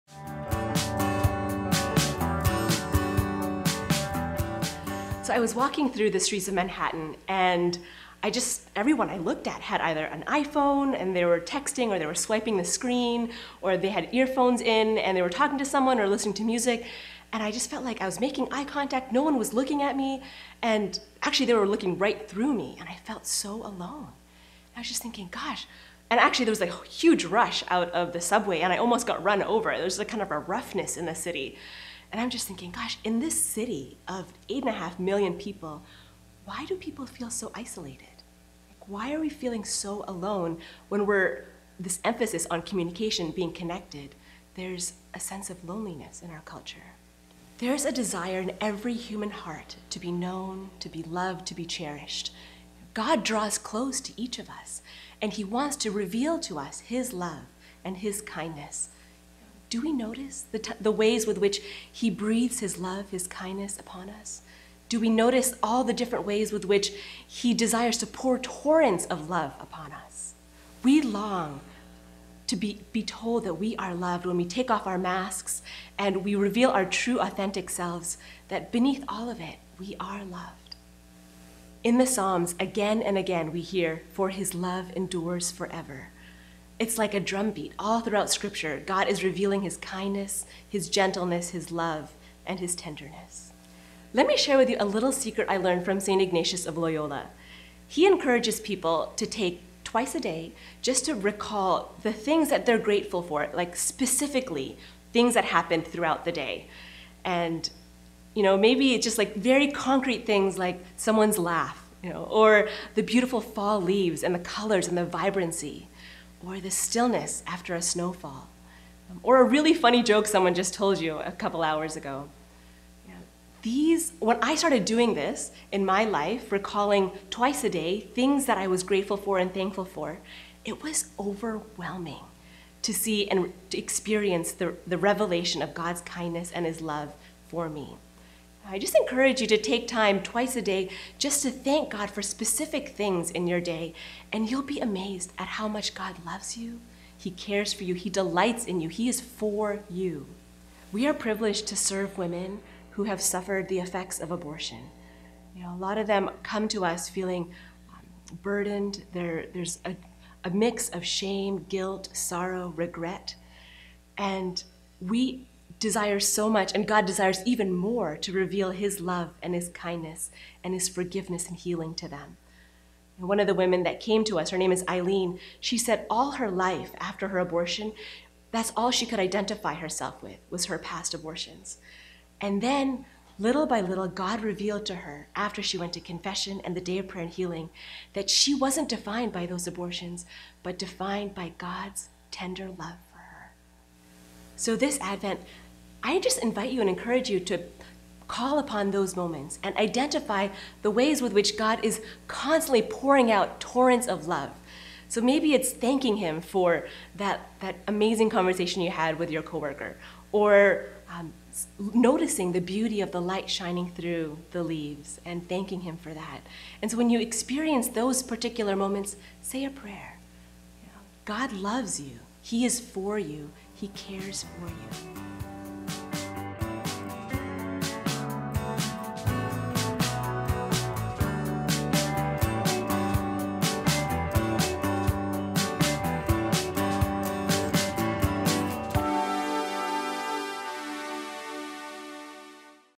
Advent ShareJesus 4: The Kindness of God
The Kindness of God with Sr. Antoniana Maria, SV

Get a daily video this Advent, join ShareJesus at adventseries.com

Thanks to Franciscan University for helping make this video series possible!